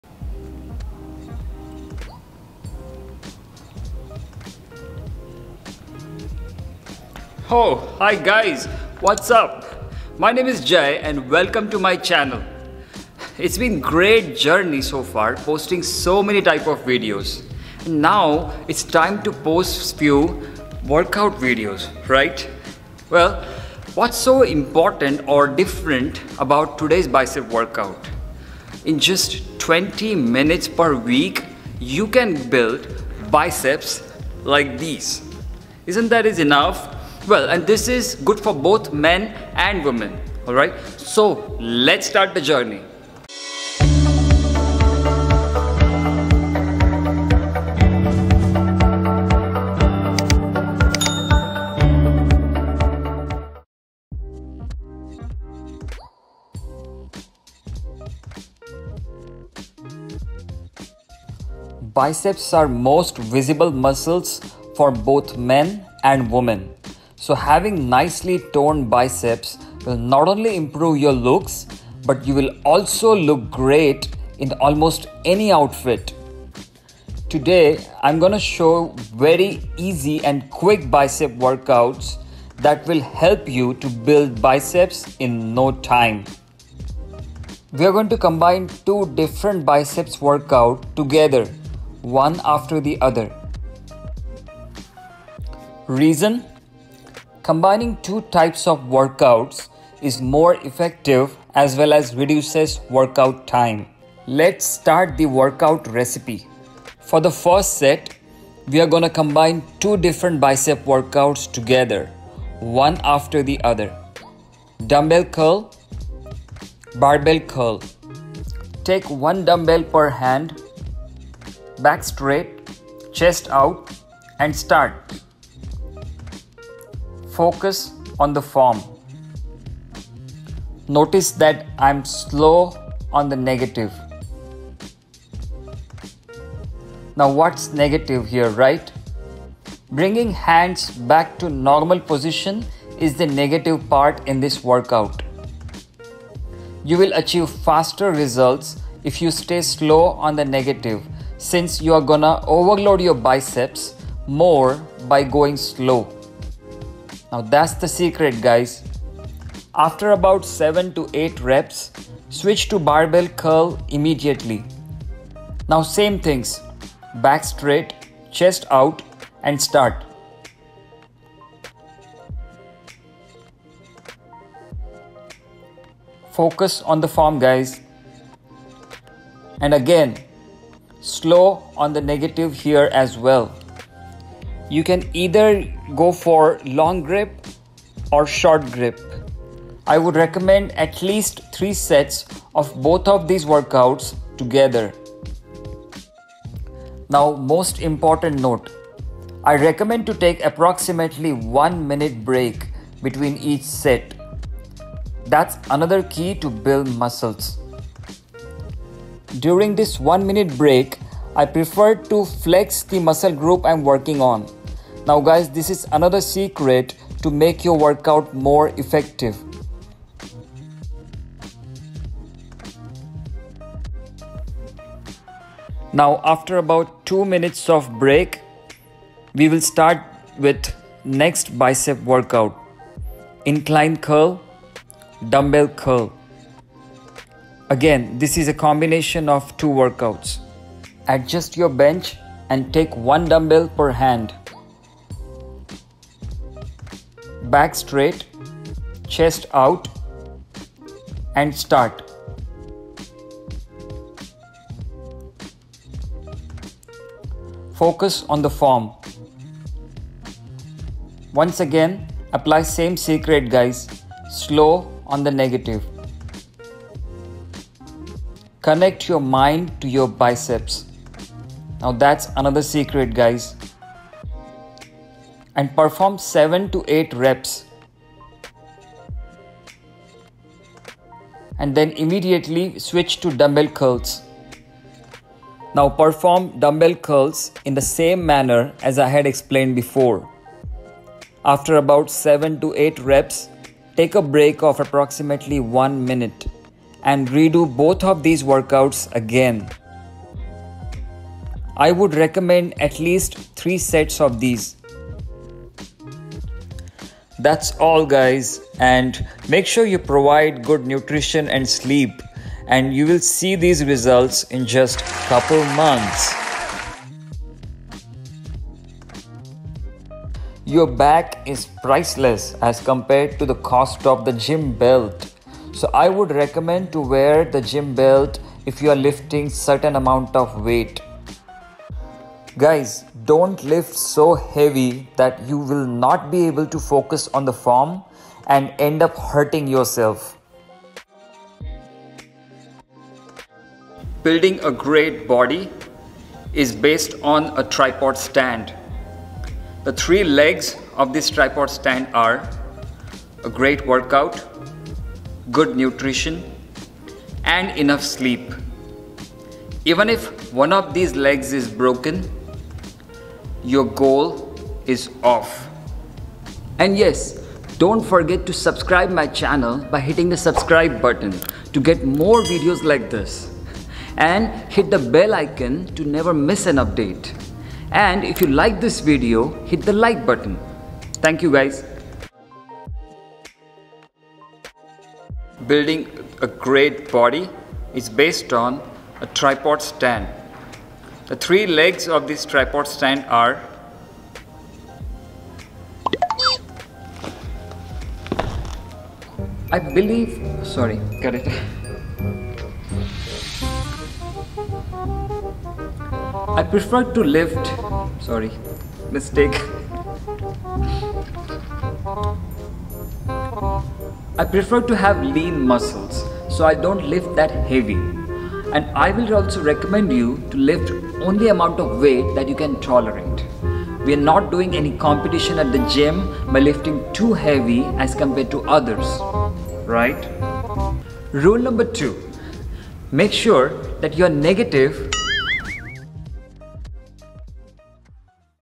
Secrets to build biceps quickly by Indian Cowboy
Hello friends,
This video illustrates awesome tricks to build nicely toned biceps by spending only 20 mins per week.

You dont have to spend hours at the gym to build biceps. With these tricks only
20 minutes per week is fine.

First workout contains two different types of biceps workouts performed one after
the other
1) Dumbbell curl
2) Barbell Curl
I would recommend minimum 3 sets of these.

Then take approximately two minutes of break and do following two one after the other.
1) Incline Curl
2) Dumbbell curl

Following are the key tricks
1) Combine two types of biceps workout together
2) Take approximately one minute break between two sets
3) Focus on the form than weight
4) Slow on the negative
5) Flex the muscles between two sets

Please comment for any questions you have and I will respond for sure.